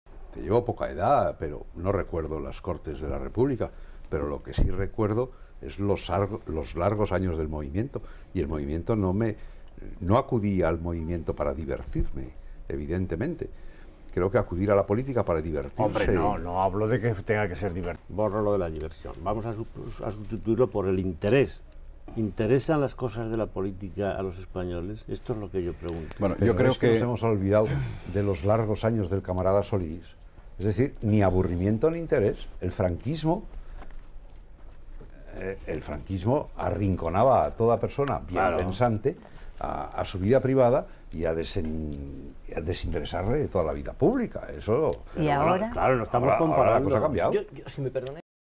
Juan Benet
El escritor Juan Benet en un debate de televisión moderado por Victoria Prego hacia 1987.